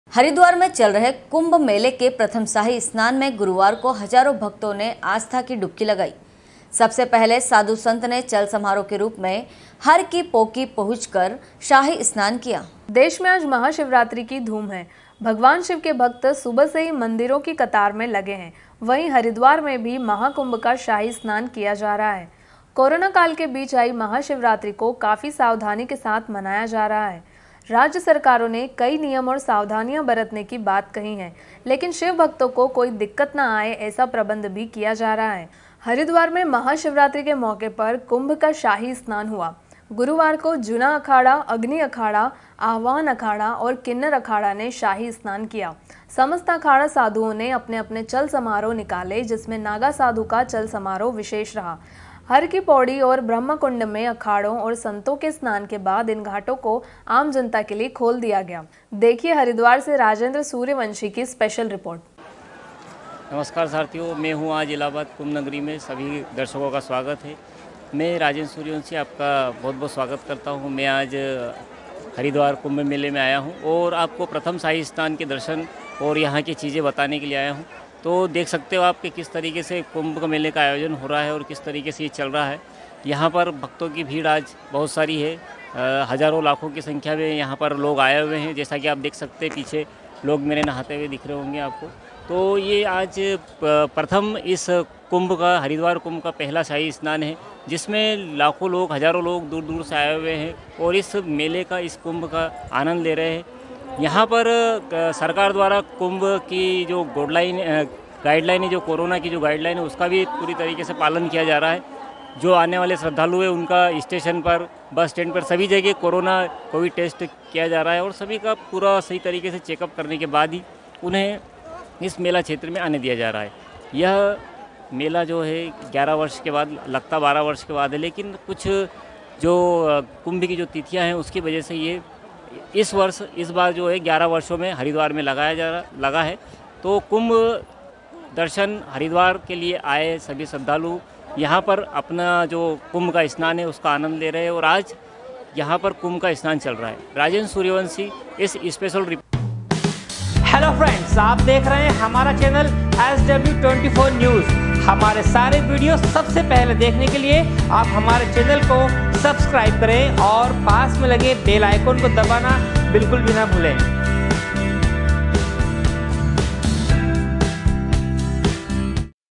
haridwar- कुम्भ मेले के प्रथम शाही स्नान में गुरुवार को हजारों भक्तों ने आस्था की डुबकी लगाई।
हरिद्वार में चल रहे कुम्भ मेले के प्रथम शाही स्नान में गुरुवार को हजारों भक्तों ने आस्था की डुबकी लगाई। सबसे पहले साधु संत ने चल समारोह के रूप में हर की पौक़ी पहुँच कर शाही स्नान किया । देखिए हरिद्वार से राजेन्द्र सूर्यवंशी की स्पेशल रिपोर्ट।